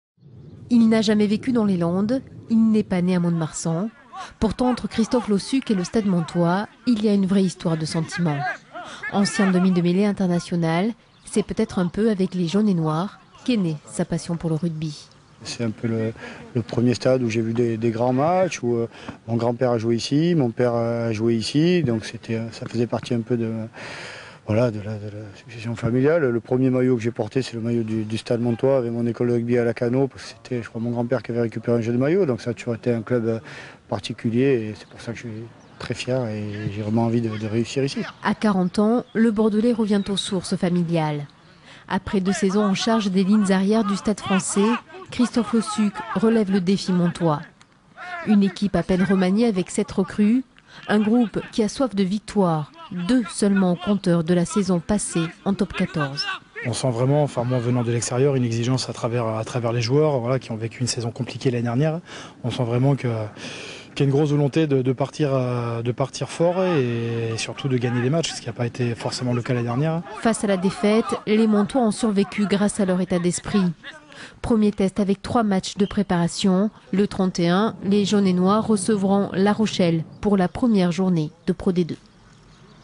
PRESENTATION CH LAUSSUCQ
Le nouveau manager général de l'équipe à l'entrainement .